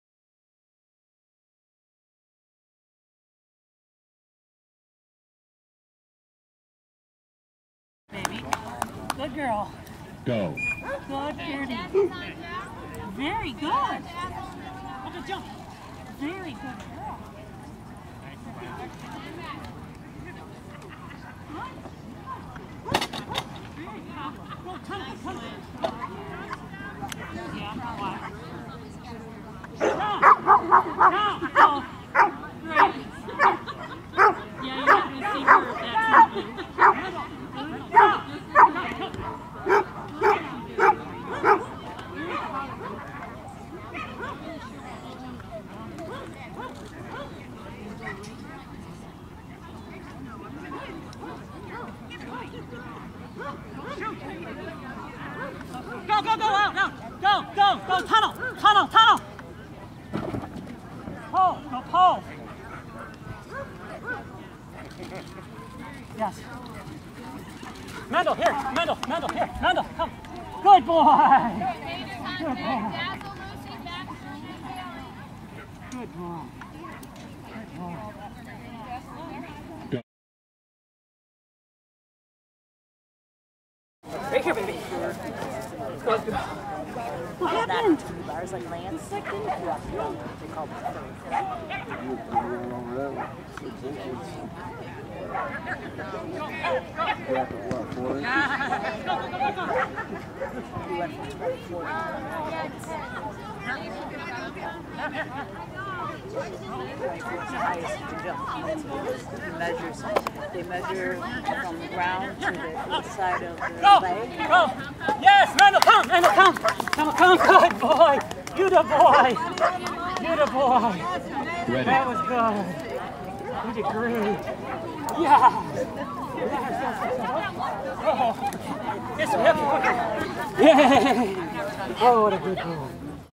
Albion AKC Agility July 25 2010
Through the weekend Mendel had good enthusiasm through the weaves and on the whole paid good attention.  His NQ's came because of a couple of knocked bars and a missed jump at the finish (he knew it was the end and prematurely went for his leash).  He is still impatient about lying down on the table... something we still need to work on!